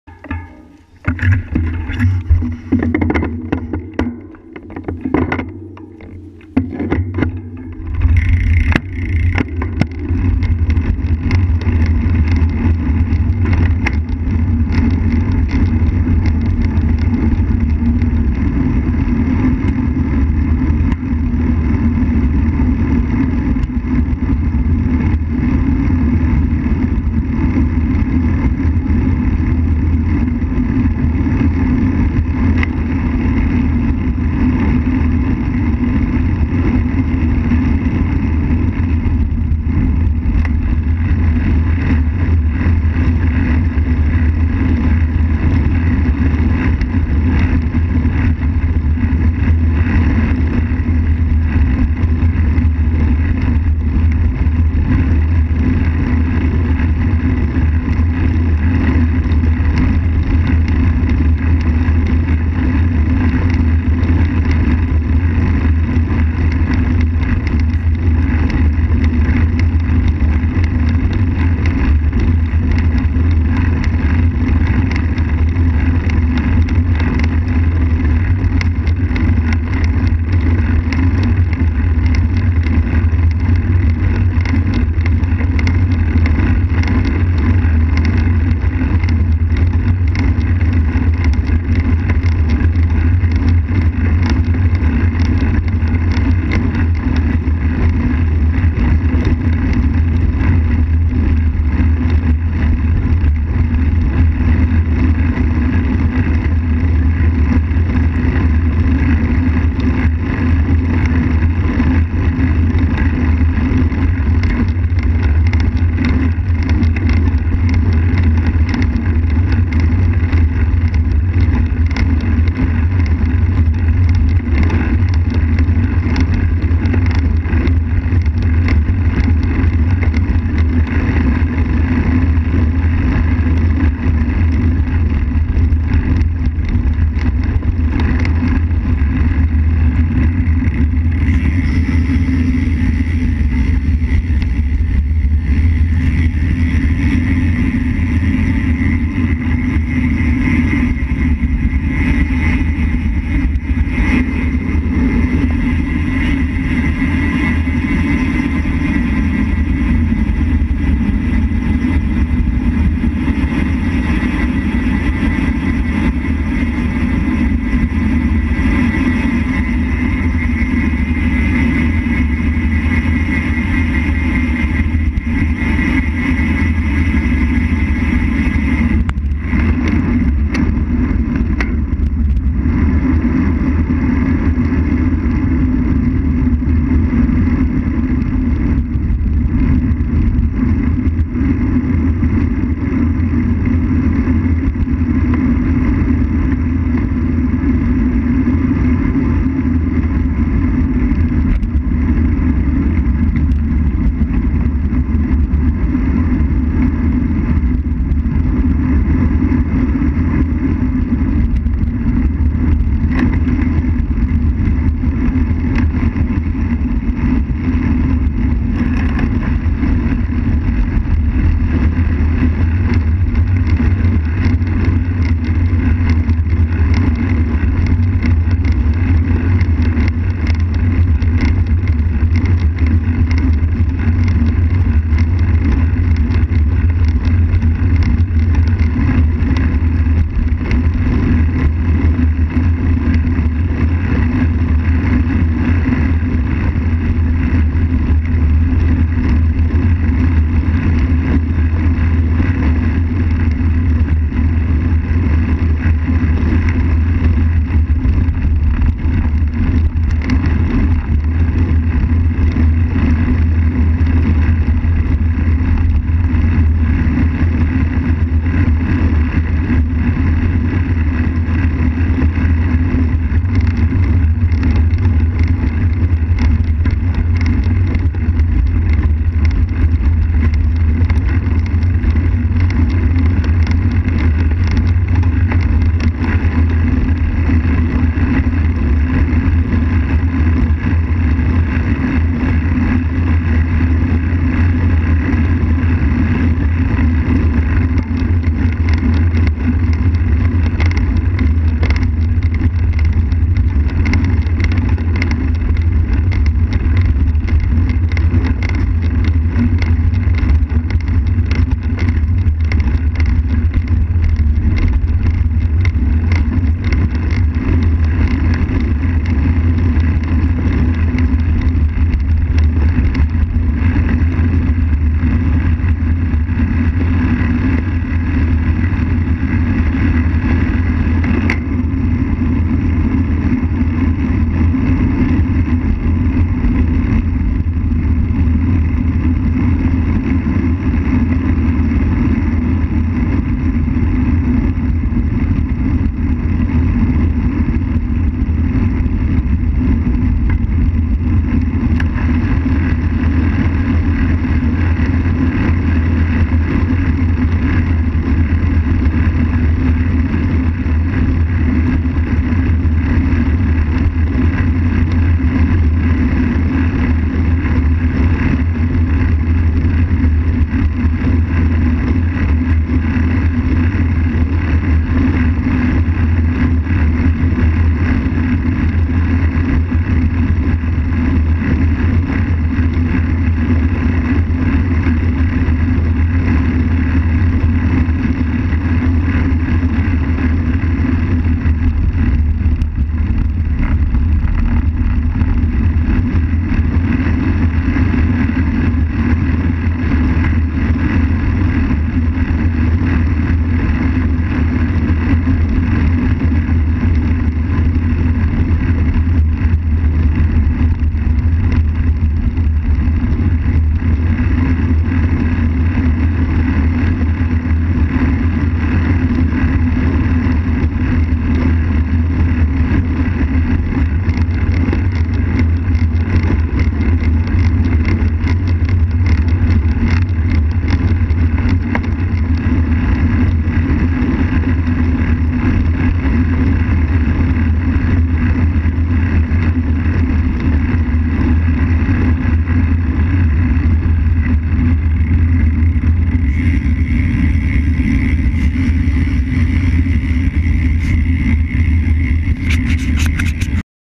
Grist Park Climb
Pops and I drop the old folks and take off on the climb out of Grist State Park.